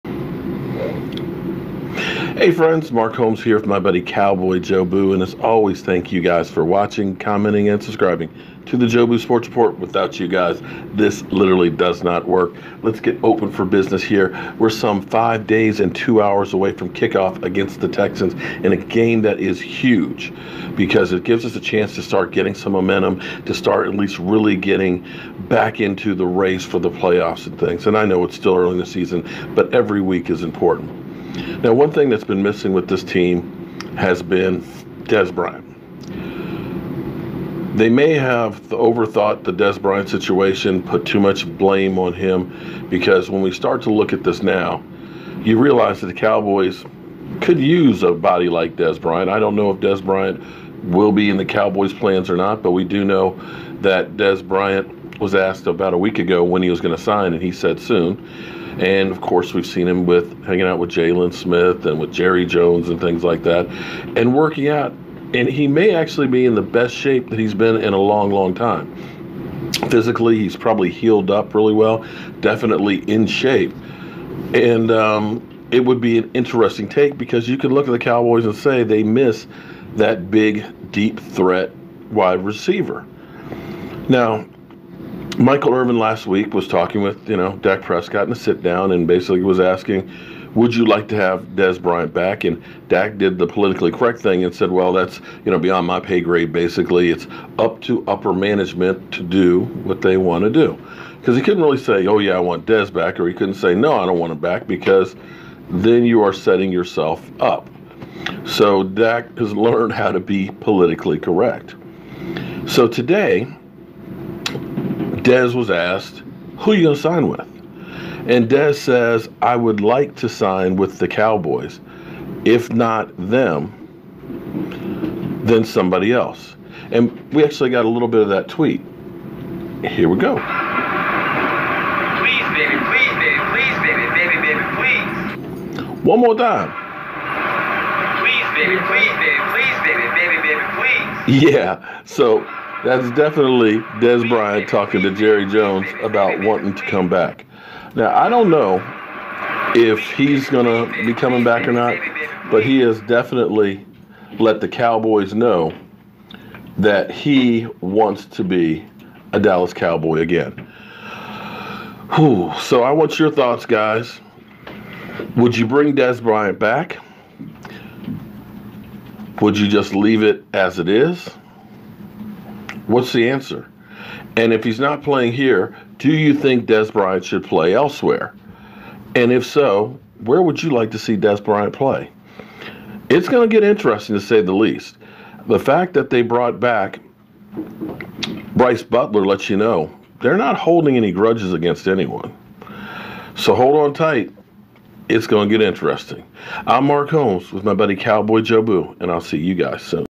Dez Bryant says he wants to come back
Dez Bryant says on twitter that he wants to come back to the Cowboys.